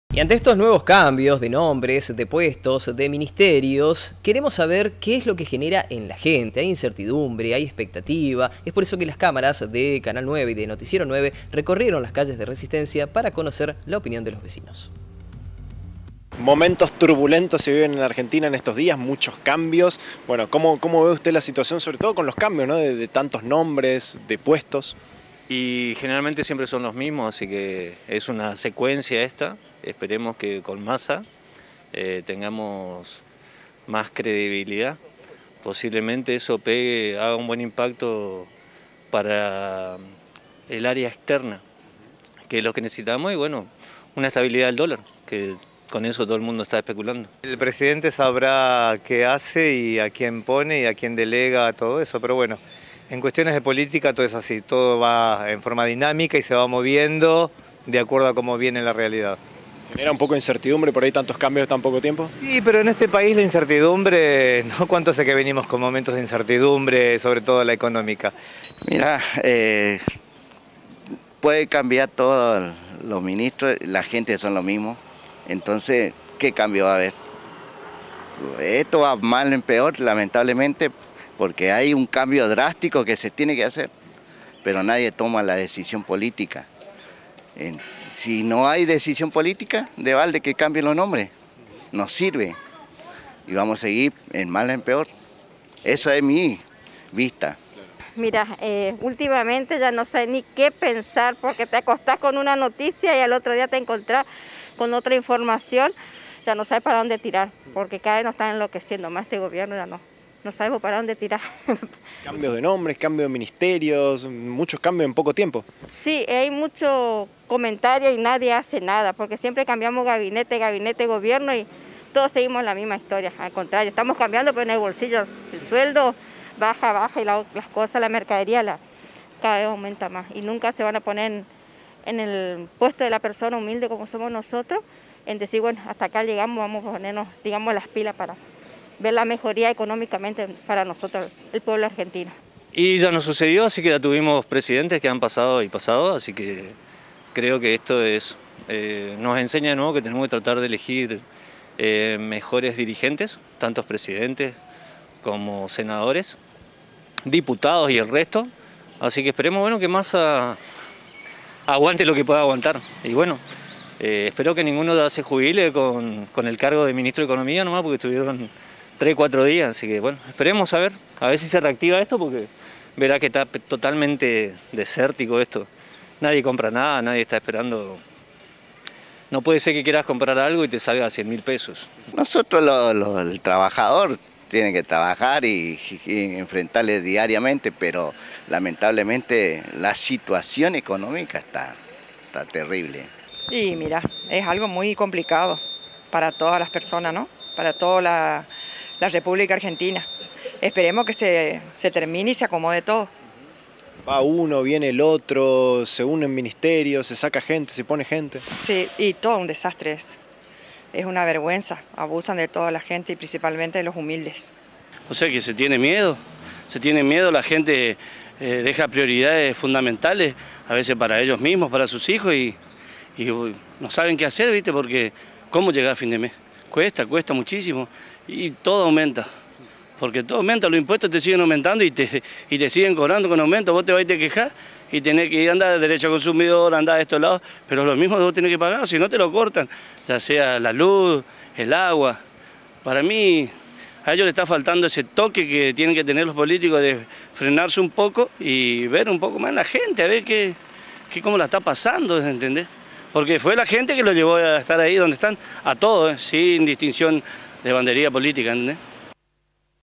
NOTICIERO 9 - CAMBIOS Y MÁS INCERTIDUMBRE - RESISTENCIA
LOS VECINOS OPINAN SOBRE LOS VAIVENES POLÍTICOS
Con horas de muchos cambios y convulsionadas a nivel político y económico, Noticiero 9 dialogó con los vecinos de Resistencia sobre los cambios de nombres,  puestos y ministerios en el gobierno nacional.  Muchos coinciden que aunque cambien cargos, siguen siendo las mismas personas,  y que la situación sigue igual o peor. Todos apuntan que tantos cambios en tan poco tiempo generan mucha más incertidumbre.